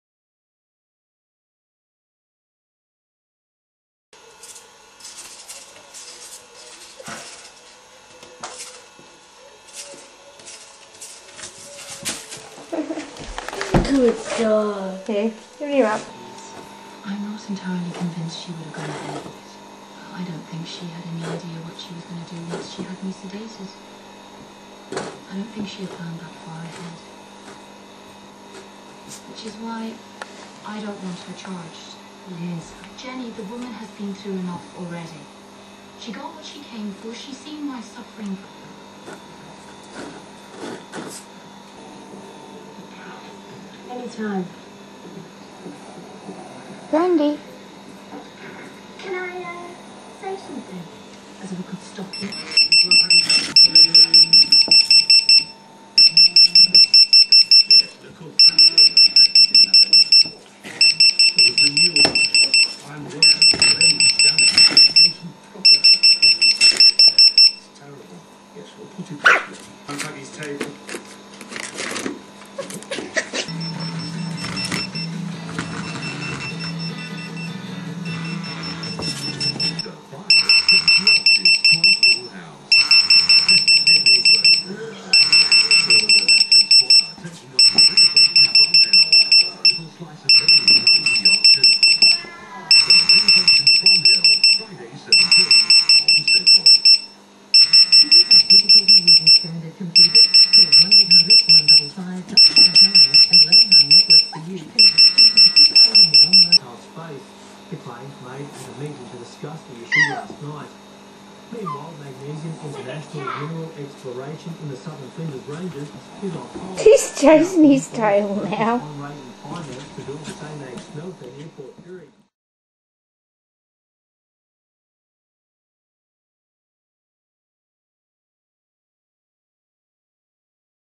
Brandy the Maltese as a Puppy he loves Mobile phone
after much searching I found the old vids of Brandy as a puppy the first night we bought him home from the breeder Isn't he the BEST :)

UPDATE 27 October 2017

My lovely boy Brandy Alexander, we got you from Debbie Hanson 14 year and 3 months ago, and you were my little version of Shannon, You bought joy to my heart when my life was almost over, you bought joy to my kids, a friend and fur brother, they spoke to you and you never complained you loved unconditionally. Today it broke my heart to say goodbye to you, our little drive to the beach, past the pound (you repeat offender you) and the back of Harris Scarfe the place where they would catch you and lock you up. You had a good life and spent 10 years missing Dion Prater it shows in the video, but age made you weary, you could no longer see, or hear, you lost your teeth and the last couple of days had some pain. I could not let you suffer, I could not be selfish, so I have placed you into Dion's care now. 3 August 2003 to 27 October 2017 RIP my boy. — with Brandy Prater.